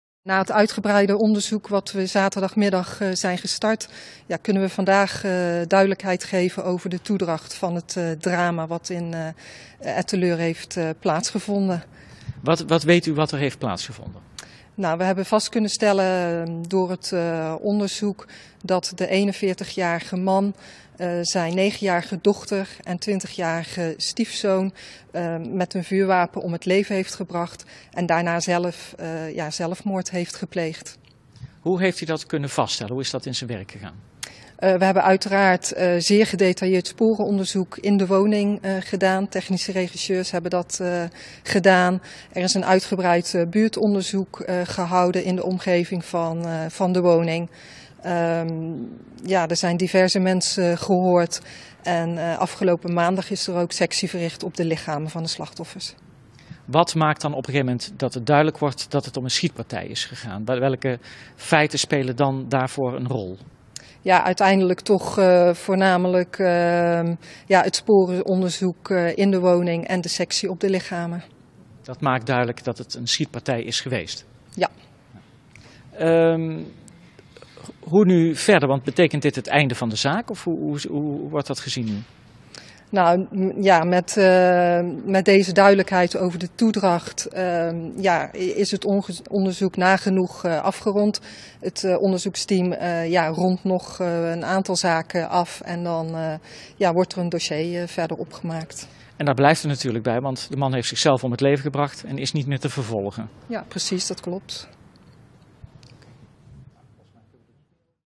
Politiewoordvoerder over onderzoek gezinsdrama Etten-Leur
Na vijf dagen onduidelijkheid, is donderdag bekendgemaakt wat er vrijdag in het huis in Etten-Leur is gebeurd. De 41-jarige Nekati Çelik heeft zijn 9-jarige dochter Melda en en zijn 20-jarige stiefzoon Jerfi doodgeschoten. Daarna pleegde hij zelfmoord.